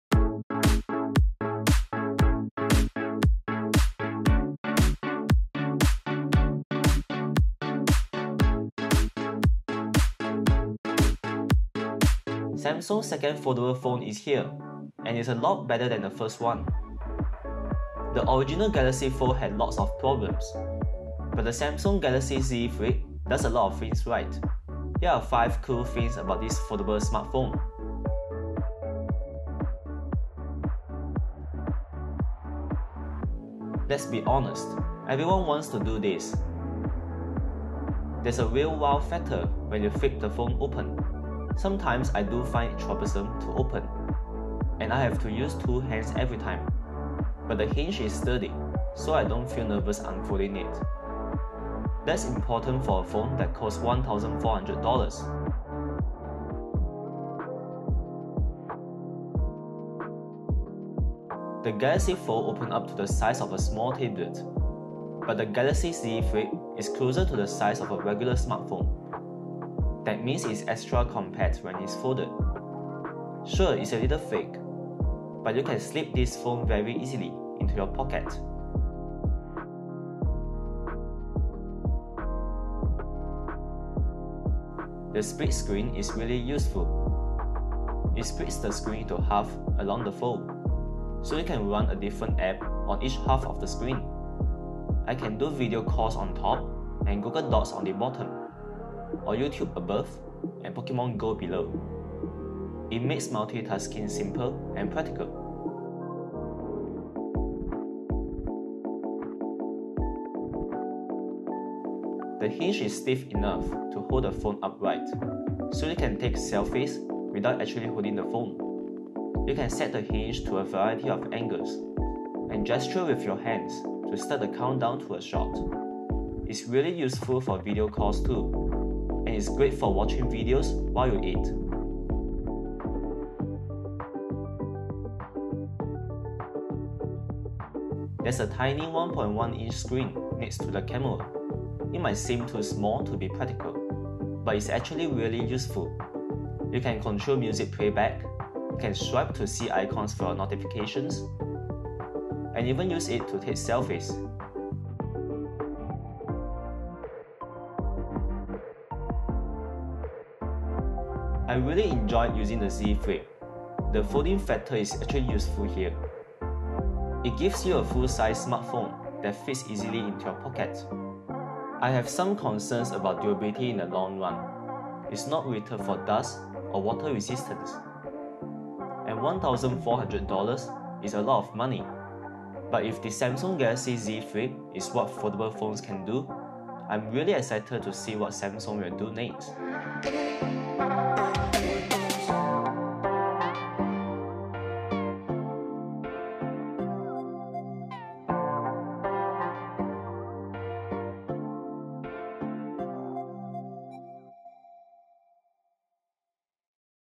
Samsung Galaxy Z Flip Review: The best foldable phone!
The Galaxy Z Flip is Samsung's best foldable phone, but that doesn't mean you should spend US$1,400 on it. But there are still some things to love about this form factor.

You can read more about the Samsung Galaxy Z Flip